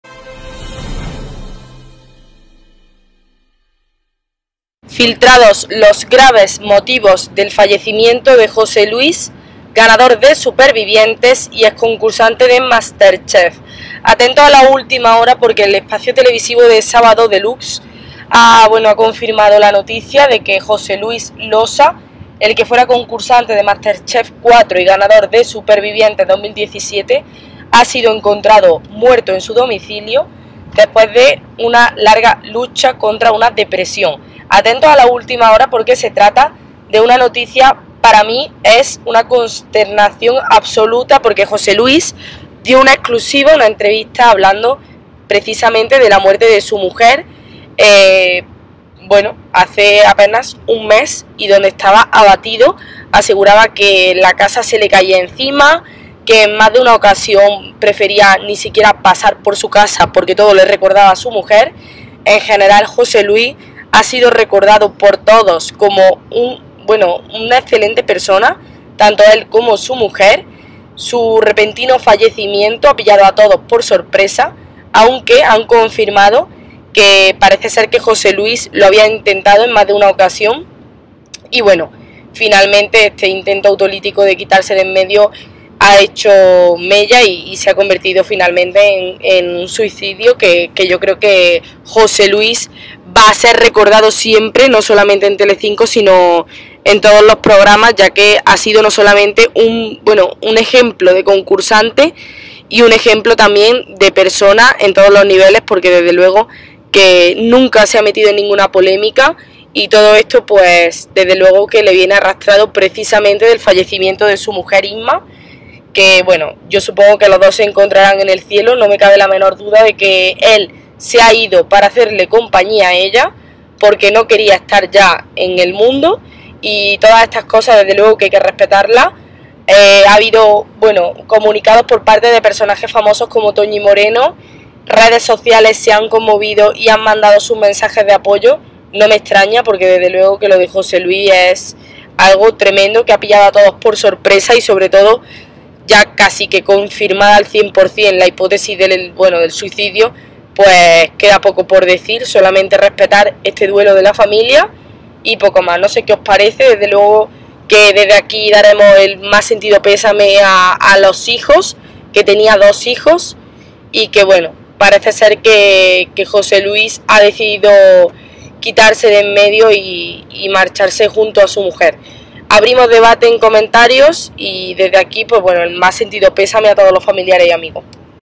⚫​TERRIBLES MOTIVOS de la muerte de José Luis de Supervivientes por Sálvame Deluxe y Telecinco
José Luis ha sido encontrado en su domicilio y han informado de su fallecimiento en Sálvame Deluxe de Jorge Javier Vázquez